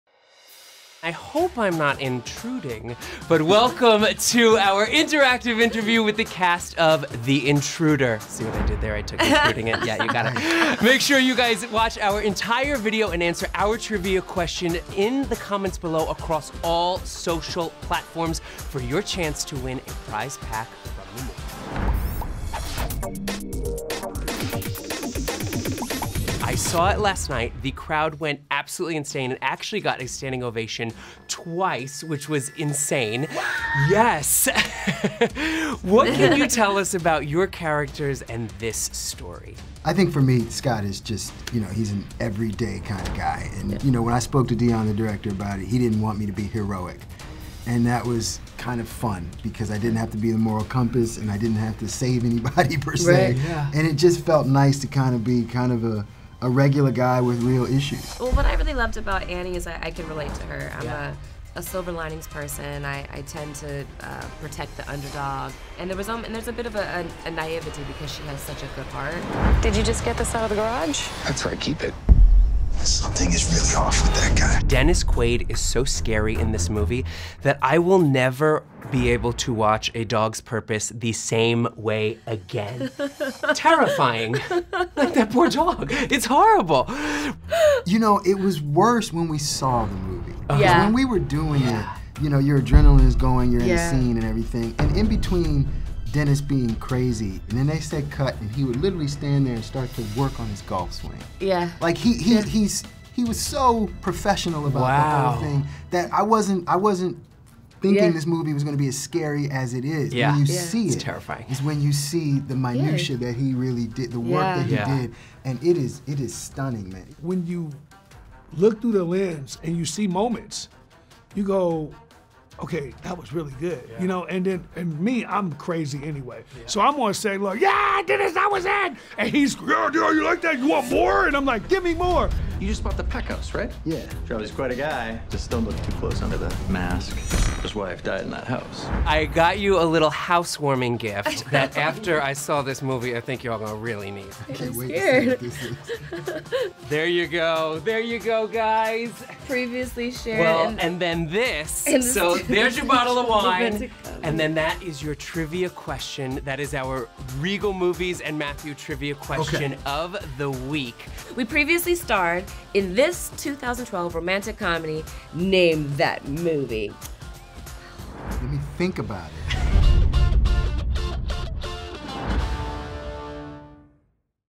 The Intruder: Sit Down with the Stars feat. Matthew Hoffmann - Regal [HD]
Our very own Matthew Hoffman sat down with the cast behind The Intruder, the new thriller coming to Regal May 3rd.

A young married couple buys a beautiful house on several acres of land only to find out that the man they bought it from refuses to let go of the property.

Director: Deon Taylor
Writer: David Loughery
Stars: Meagan Good, Dennis Quaid, Michael Ealy